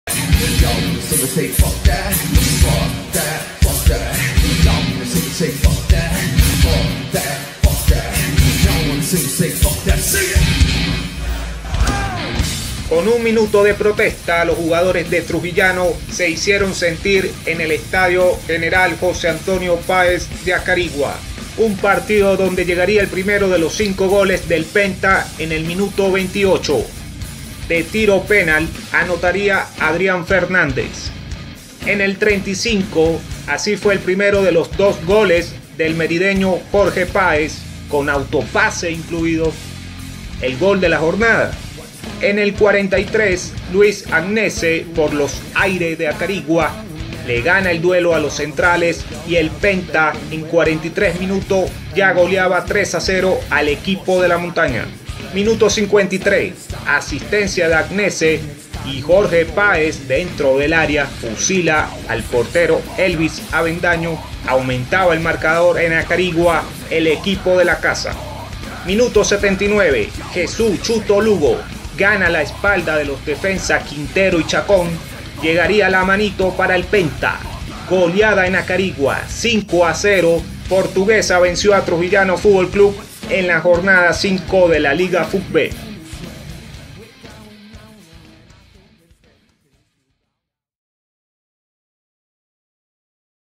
Portuguesa goleó a un golpeado Trujillanos Fútbol Club.
Futve | Jornada 5